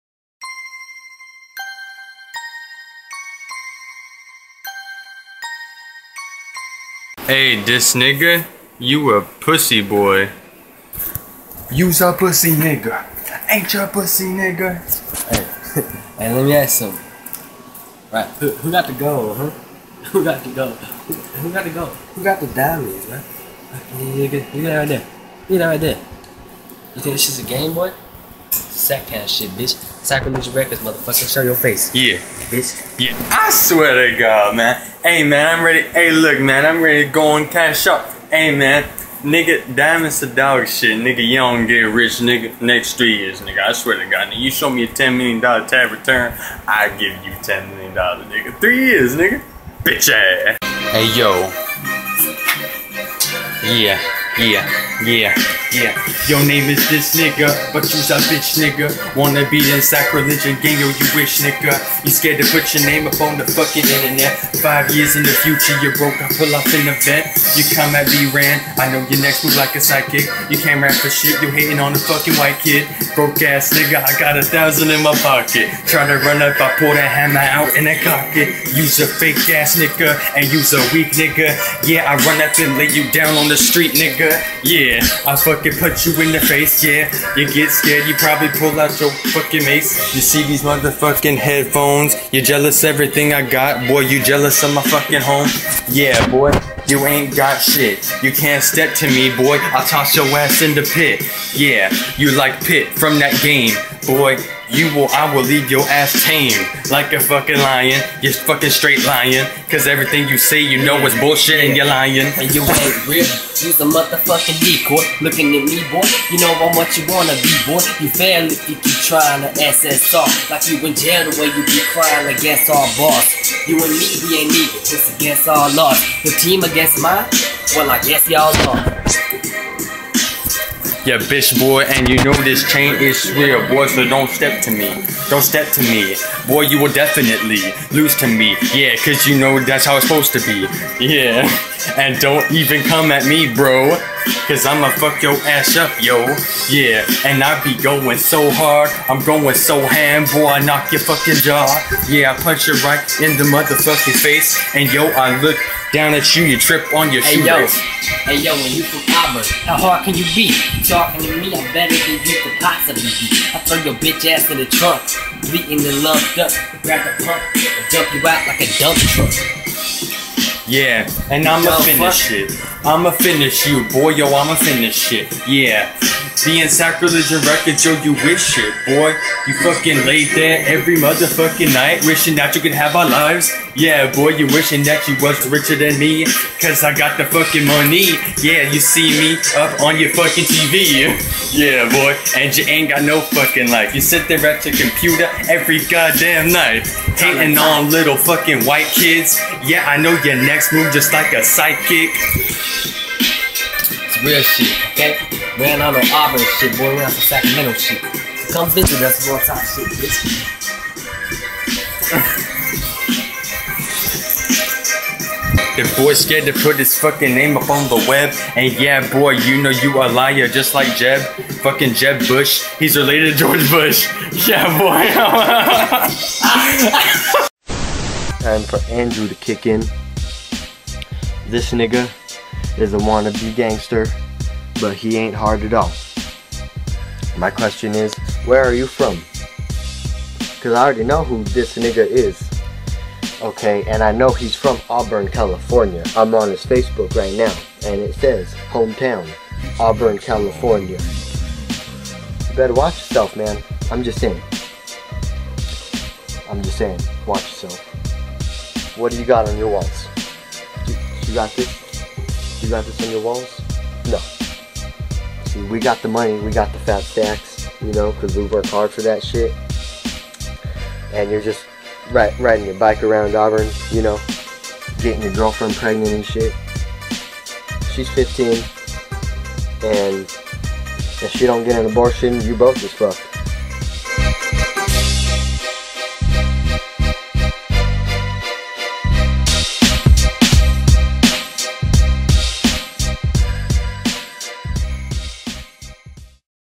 "This Nigga" Gets Exposed by B-Ran, KevvBundy & Androo
"This Nigga" gets shit on by the SacReligion squad